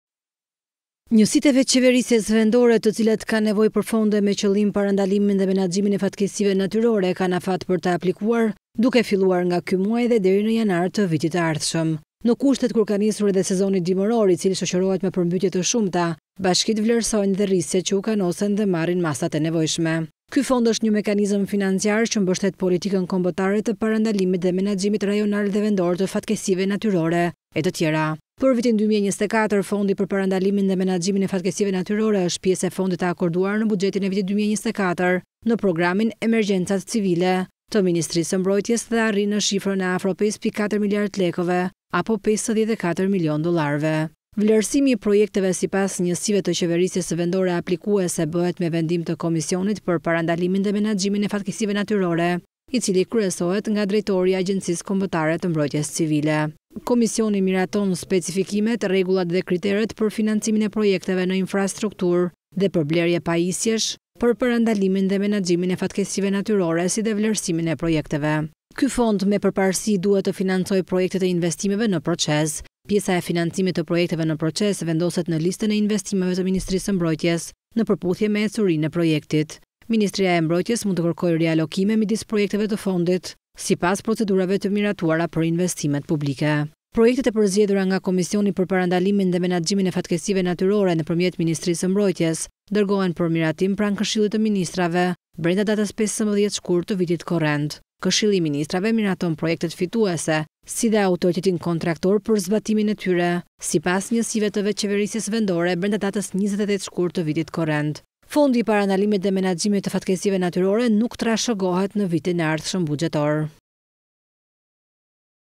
Hapen aplikimet për fondin e fatkeqësive natyrore – 54 milionë dollarë drejt bashkive për përmbytjet
Njësitë e vetëqeverisjes vendore të cilat kanë nevojë për fonde me qëllim Parandalimin dhe Menaxhimin e Fatkeqësive Natyrore kanë afat për të aplikuar duke filluar nga ky muaj dhe deri në Janar të vitit të ardhshëm.

Në kushtet kur ka nisur edhe sezoni dimëror i cili shoqëroret me përmbytje të shumta, bashkitë vlerësojnë edhe rrisqet që u kanosen dhe marrin masat e nevojshme. Ky fond i është një mekanizëm financiar që mbështet politikën kombëtare të parandalimit dhe menaxhimit rajonal dhe vendor të fatkeqësive natyrore e të tjera.

Për vitin 2024, Fondi për Parandalimin dhe Menaxhimin e Fatkeqësive Natyrore është pjesë e fondit të akorduar në buxhetin e vitit 2024 në programin “Emergjencat Civile”, në Ministrinë e Mbrojtjes dhe arrin në shifrën e afro 5.4 miliardë lekëve apo 54 milionë dollarëve.